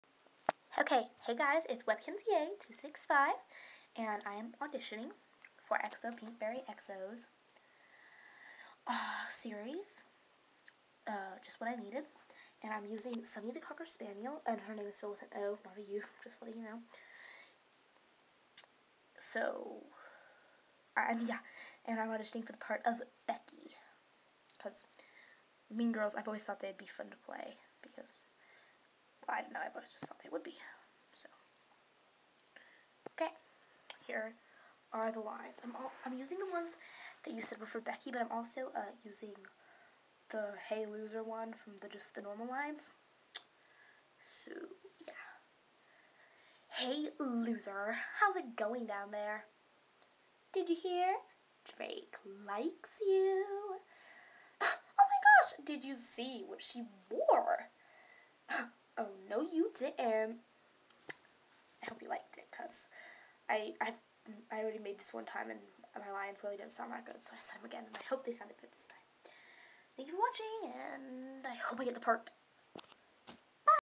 Audition for xoPinkBerryxo's Just What I Needed series
I am auditioning for the part of Becky and my Webkinz is Sonny the Cocker Spaniel.